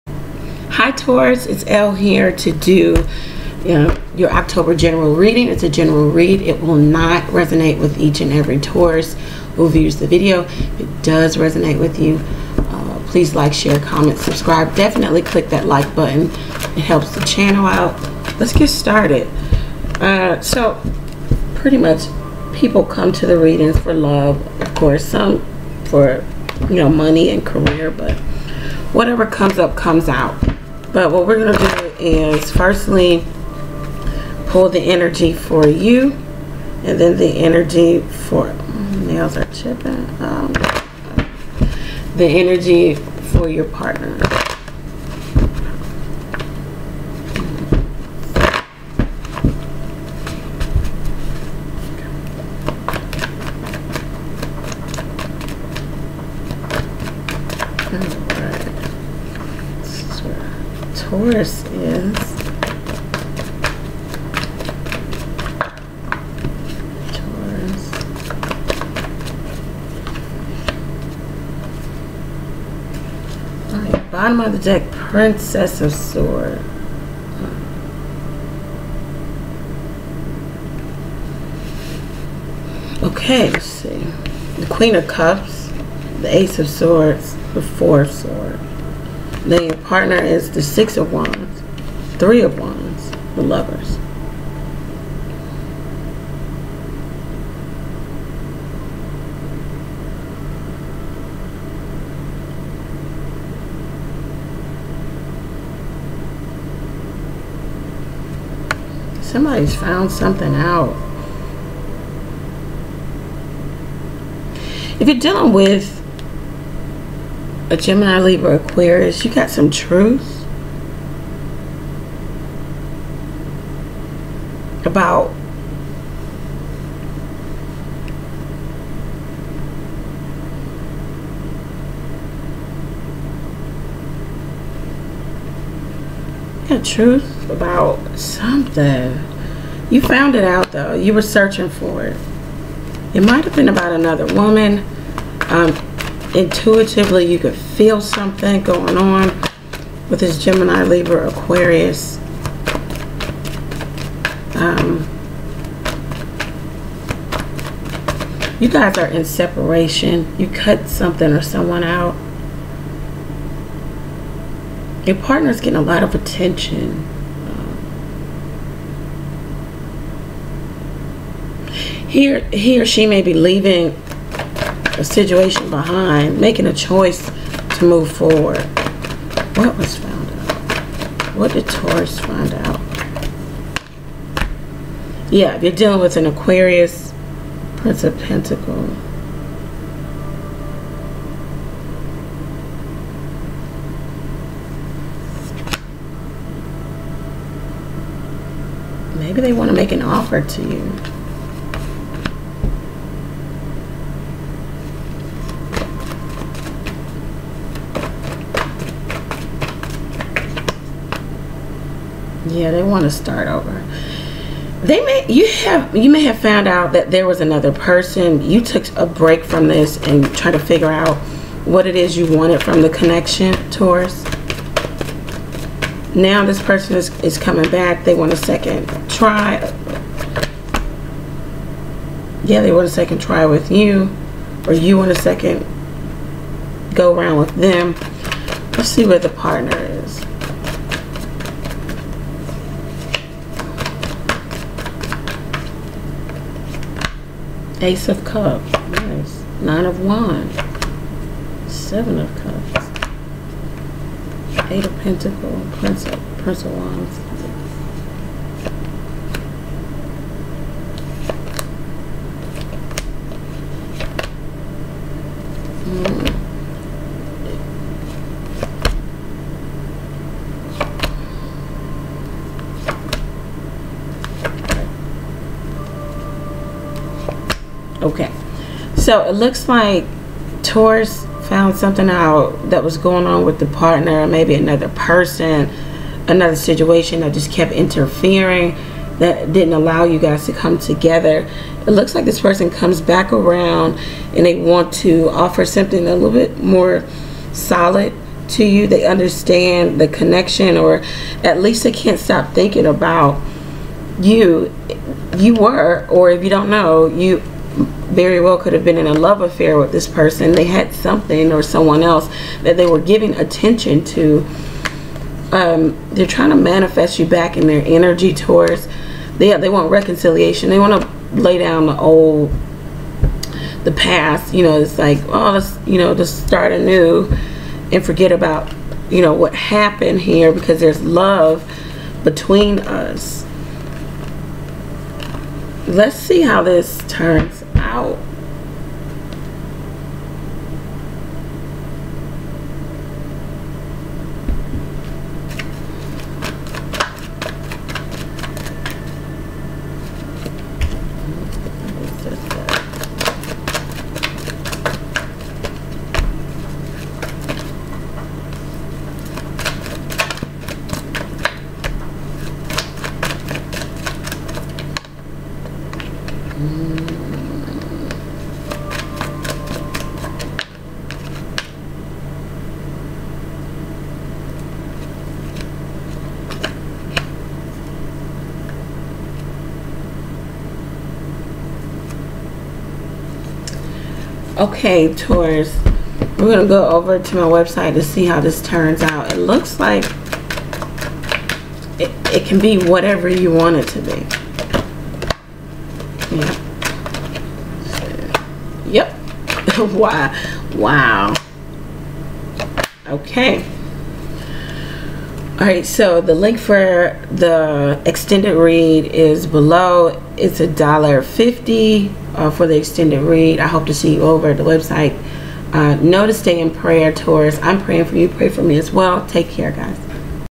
TAURUS OCT 2018 YOU FOUND OUT ABOUT THE OTHER PERSON
My astrological makeup:

Sun: Aquarius
Moon: Libra
Ascendant: Aries

Ellej446
TAROTOFELLE